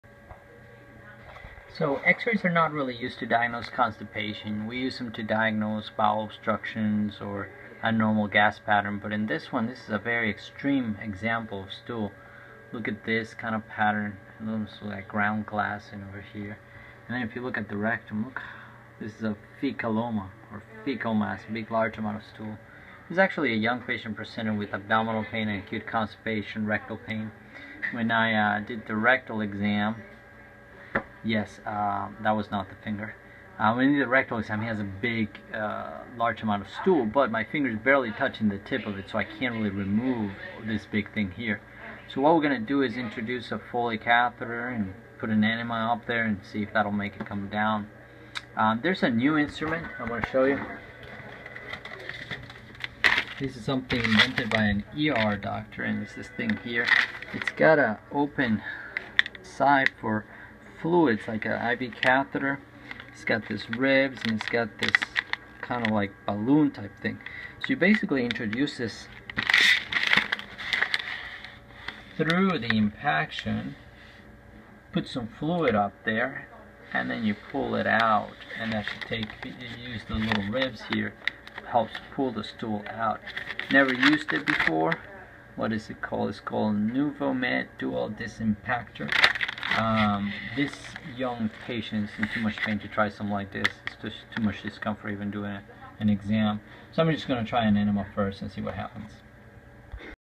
Fecal impaction
this young patient presented with severe rectal pain and pressure and inability to pass his bowels for a few hours before presenting to the ED. I did a rectal exam that showed that indeed had a large rectal impaction but because of my 'short' fingers I am unable to manually pull out. X-ray ordered and performed before I saw the patient confirms a big 'fecaloma' and abundant stool throughout the colon.

I am going to use this 'new' device invented by an ER doctor to manually disimpact these obstructions using a plastic cathter with water ports (helps lubricated and break up the stool) and then pulls out the stool as the device is withdrawn from the rectum.

After the procedure the patient bought me a drink! he he he
not really, just kidding.